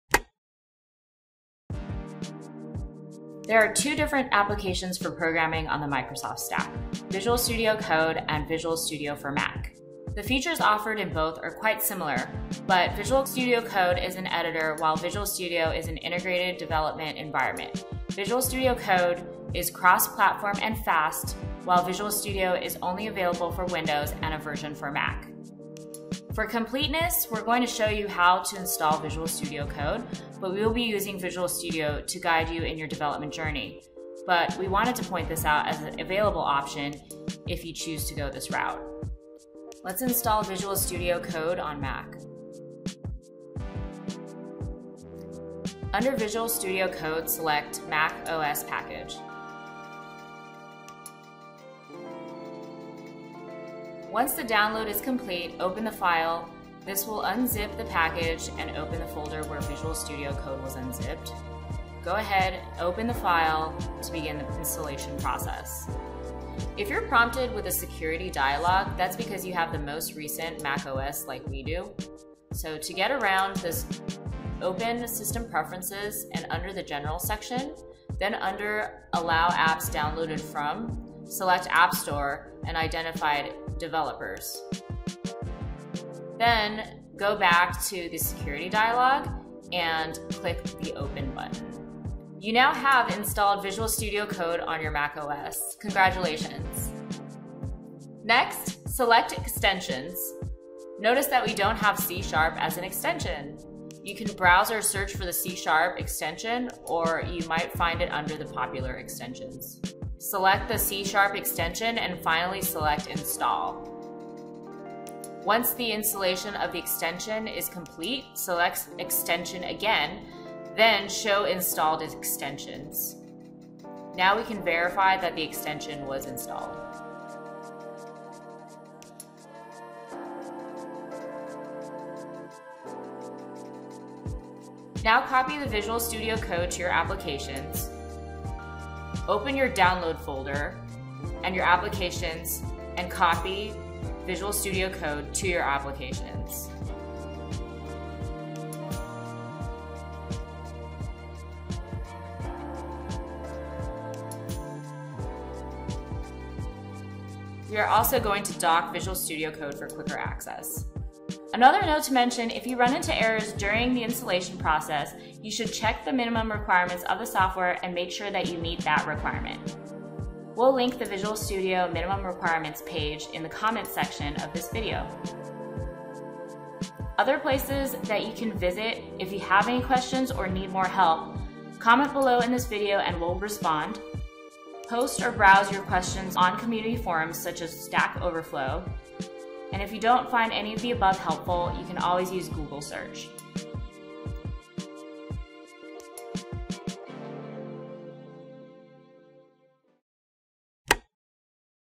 Part 4 - Installation of Visual Code for Mac
Tutorial for installing Visual Studio Code for Mac.